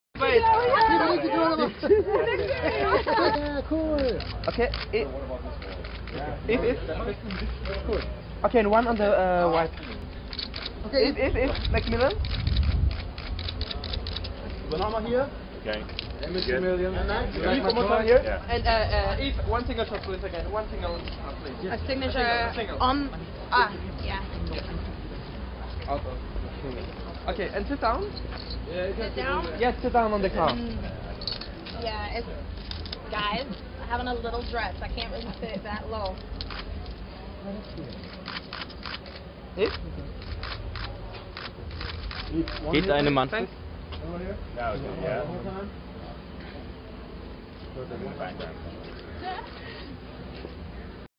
Rapperin Eve posiert mit heißen Öfen
Rapperin Eve ließ es sich nicht nehmen, mit den heißen Flitzern des Gumball3000 zu posieren.

Veröffentlicht bei TIKonline.de am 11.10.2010